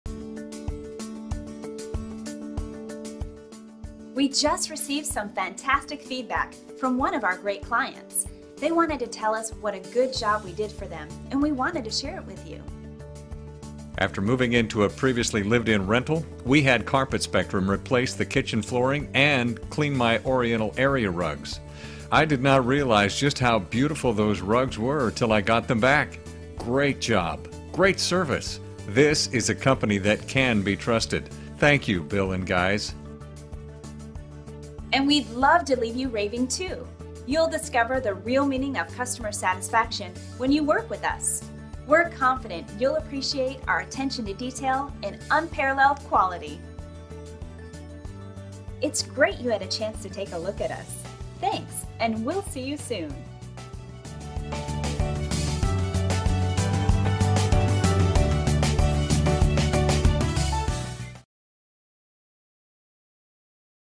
Carpet Spectrum Memphis Amazing Five Star Review by S. H.
New Review

After moving into a previously lived in rental, we had Carpet Spectrum replace the kitchen flooring and clean my oriental area rugs.  I did not realize just how beautiful those rugs were til I got them back!  Great job!  Great service!  This is a company that can be trusted!  Thank you Bill and guys!

Carpet Spectrum
3702 Jackson Avenue
Memphis TN
38108